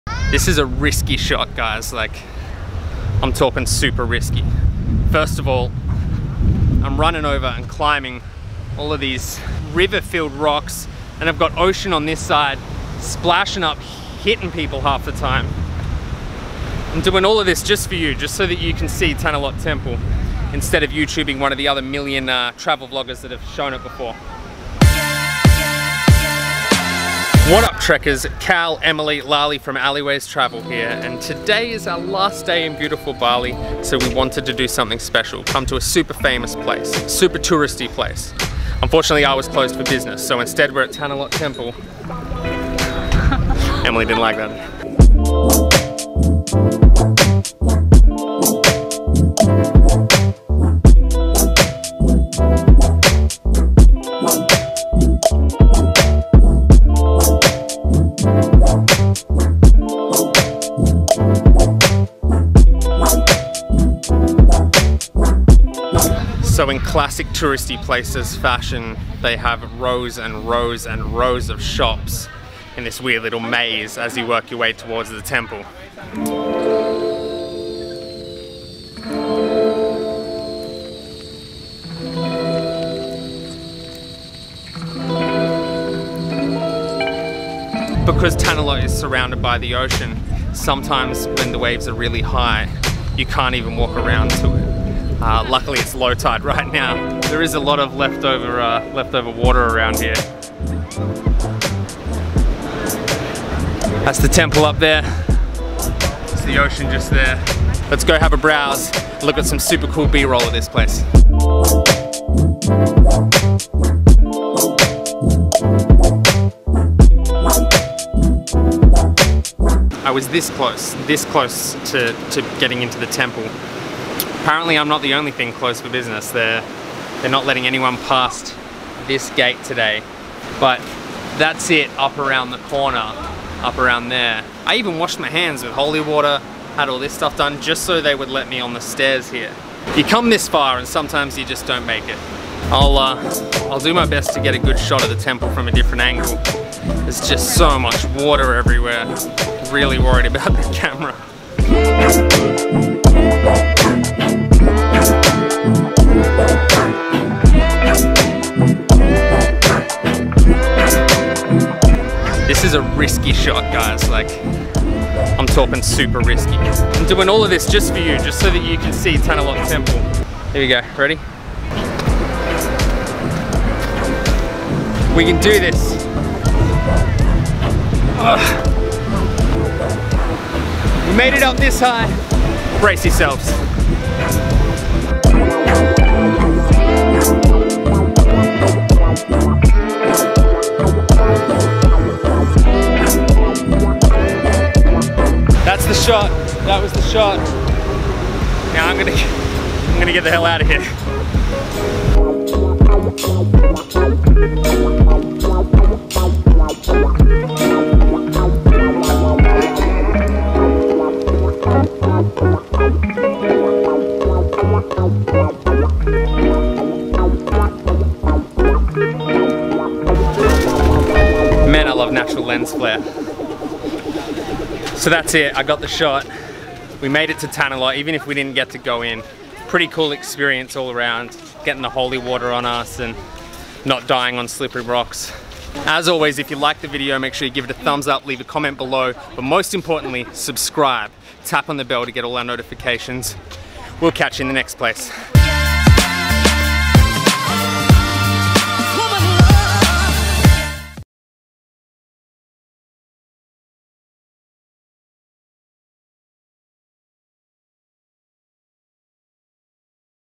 Nobody Can Enter Tanah Lot Sea Temple in Bali
Tanah Lot is a rock formation off the Indonesian island of Bali. It is home to the ancient Hindu pilgrimage temple Pura Tanah Lot, a popular tourist and cultural icon for photography.
We explore the famous temple and its surrounds in this travel vlog on our last day in Bali. Watch as Cal climbs rocks and avoids ocean spray, trying to get a good shot of the temple at sunset.
Note: Entrance to the temple is never open to the public, though you can walk around it and climb it's steps during low tide.

The Camera
The Tripod
The Microphone
The Drone
The Laptop
The Editing Guide
The Travel Guide